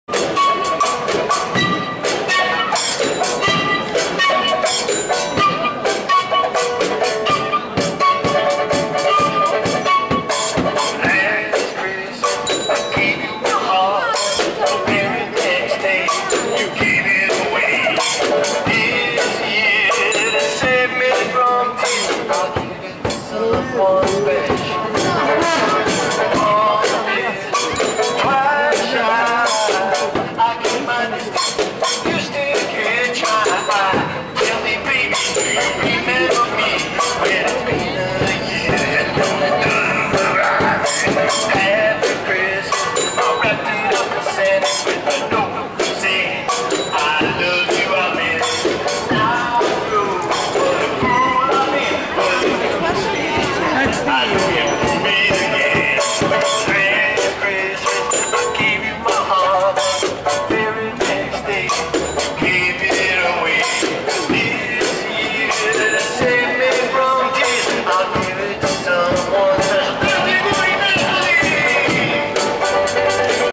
NANO TV - NAPOLI 2 GENNAIO 2011 CON I SALDI ARRIVANO ANCHE MUSICA E GENTE IN VIA TOLEDO.MPG
LEGGI PIU' NOTIZIE SU NAPOLINORD.IT  E  VOXNEWS.IT